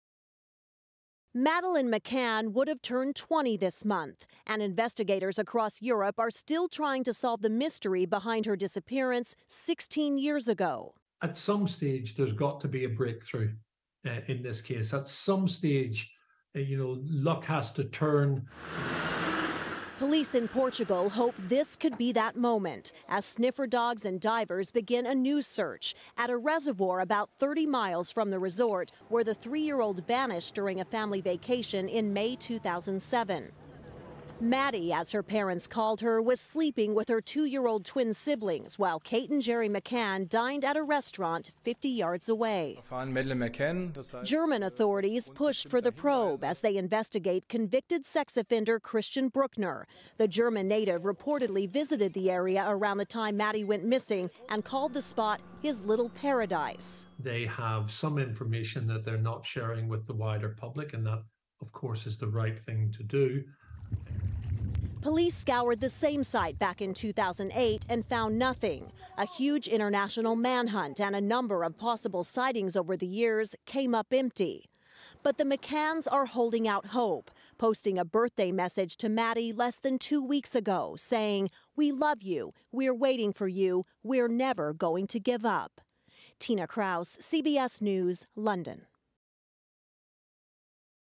New search for Madeleine McCann in Portugal
New information has led police to scour the area around a reservoir in Portugal for the remains of Madeleine McCann, a British girl who disappeared from a resort in 2007. CBS News' Tina Kraus has the latest information.